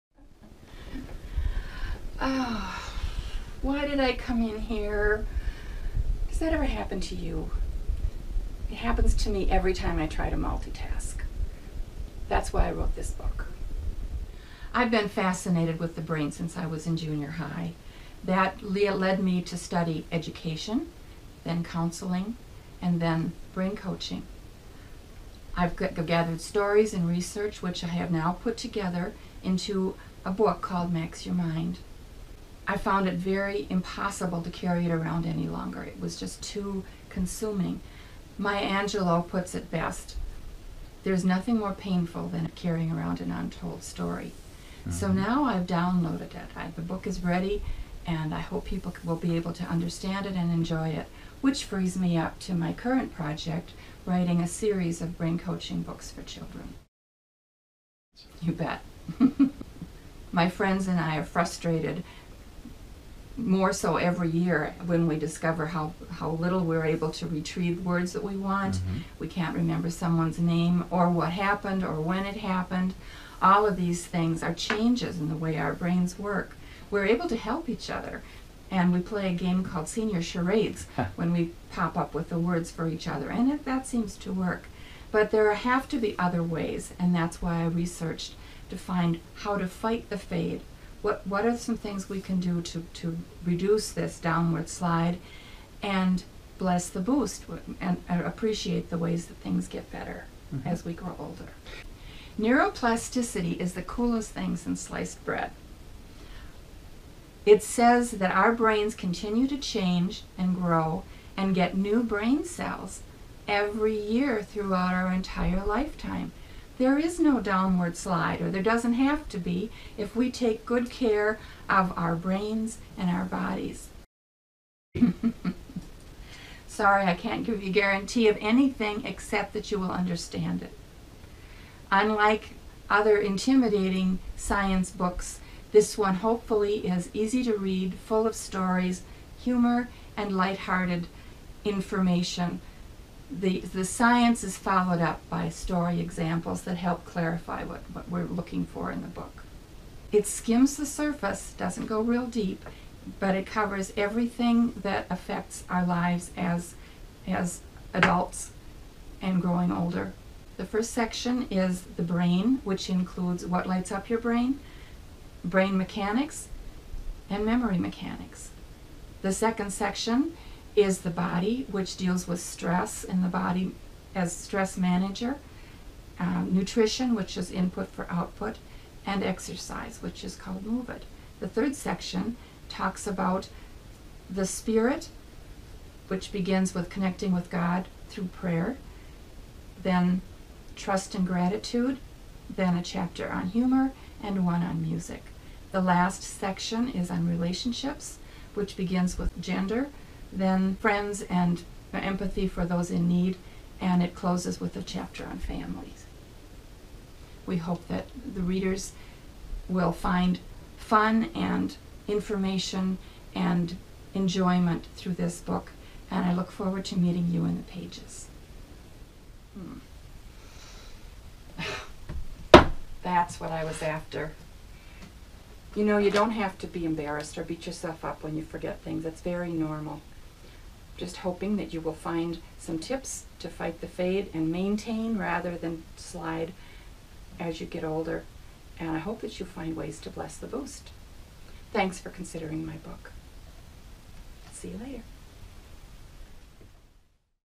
Why Did I Come Into This Room?
Teaser for Sandra Sunquist Stanton's new book, 'Max Your Mind, The Owner's Guide for a Strong Brain.' Sandra explains why the book was written and how it can help the reader retain a sharp mind with a bit of brain and body care-taking.